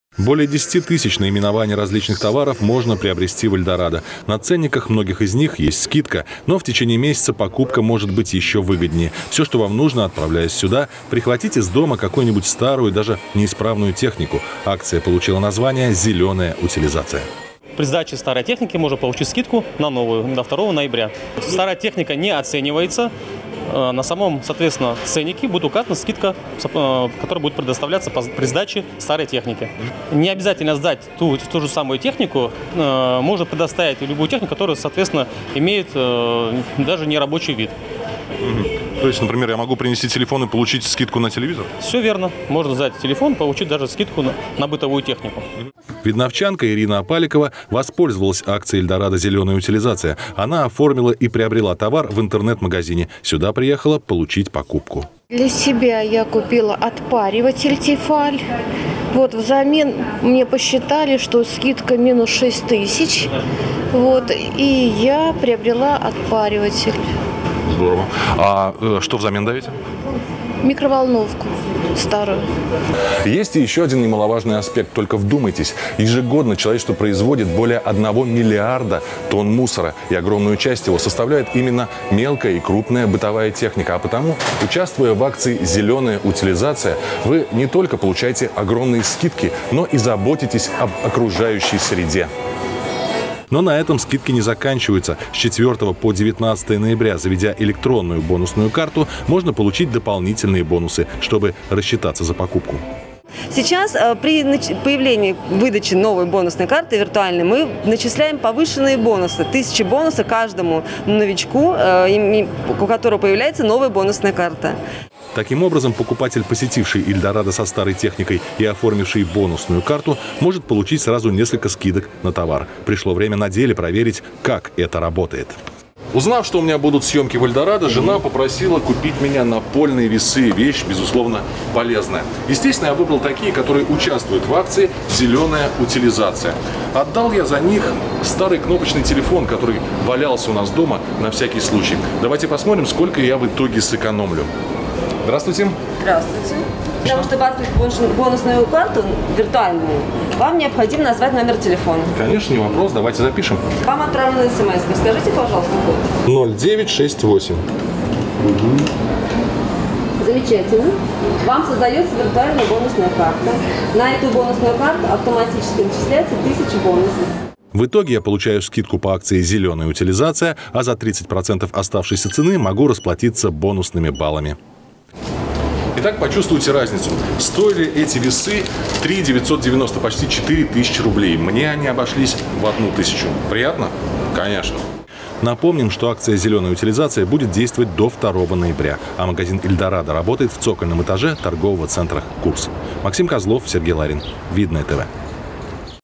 «Зеленая утилизация» - акция магазина «Эльдорадо»
Можно ли делать выгодные покупки и одновременно заботиться о окружающей среде? Как ни странно звучит ответ на этот вопрос, но он положительный. Убедиться в этом можно, посетив магазин «Эльдорадо», расположенный в торговом центре «Курс».